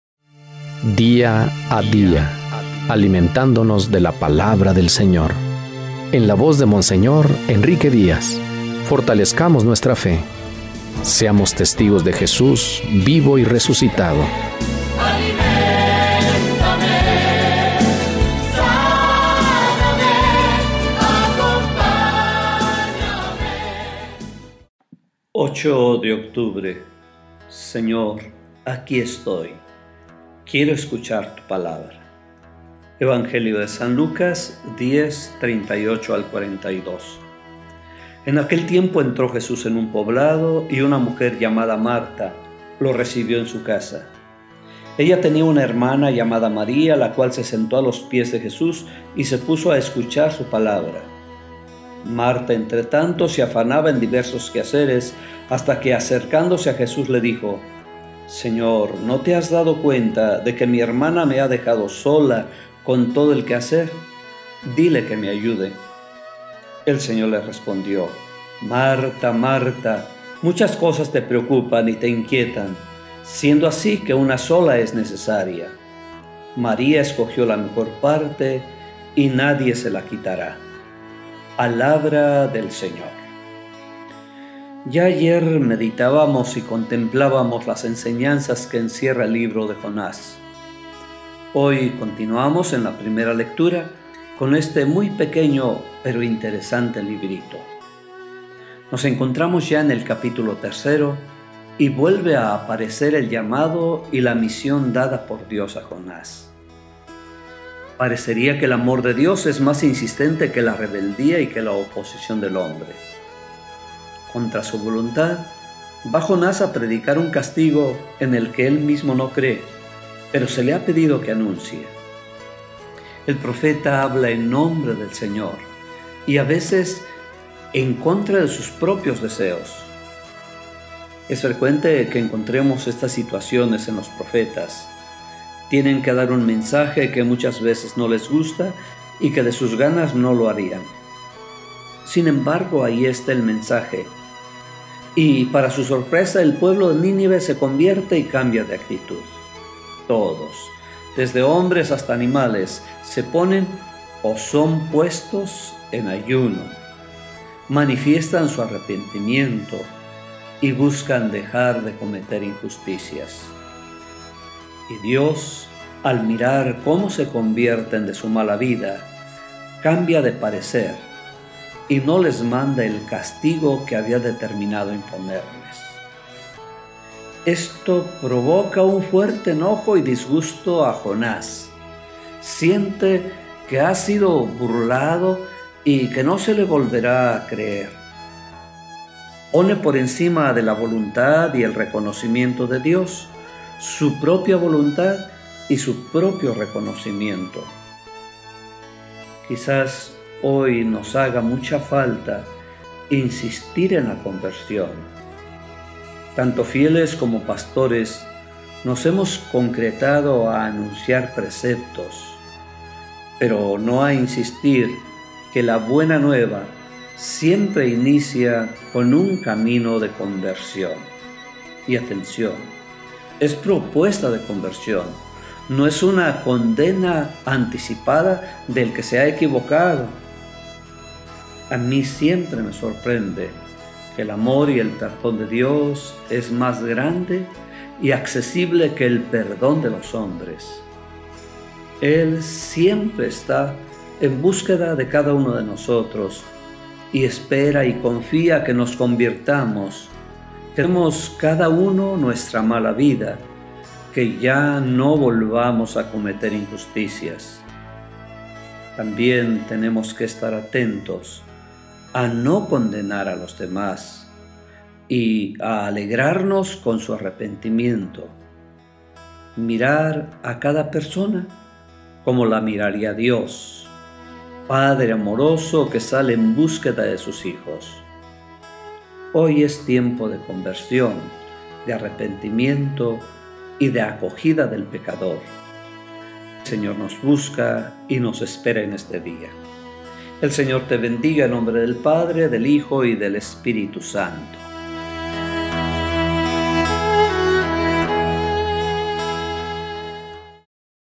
El Evangelio del Día | Mons. Enrique Díaz | 08 de Octubre de 2019
San Lucas 10,38-42: “María escogió la mejor parte y nadie se la quitará”